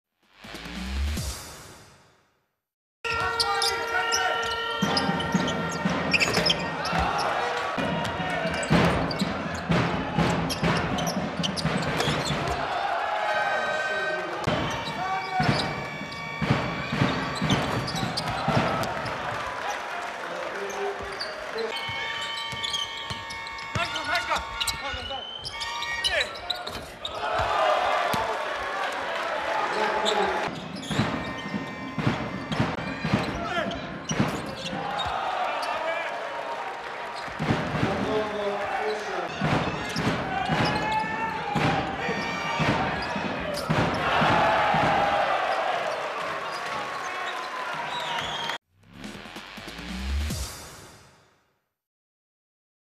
Marko Kešelj had a pretty good shooting night (Krka - Crvena zvezda mts, 4.1.2018)
Crven zvezda mts forward scored 6 three-pointers out of 7 attempts and on top of that he was the team's top scorer with 20 points in the match against Krka.